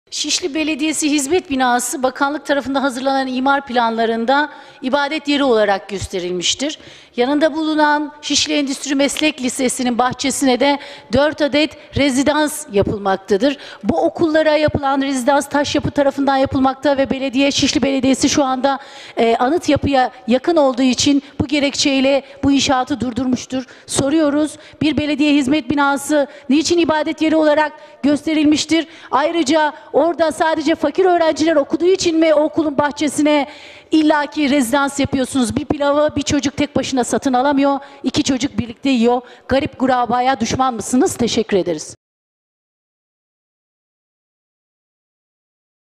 Garip Gurebaya Düşman Mısınız?
İstanbul Milletvekilimiz Gülay Yedekçi: İbadet yeri adı altında her alanı rantınıza alet etmeye çalışıyorsunuz.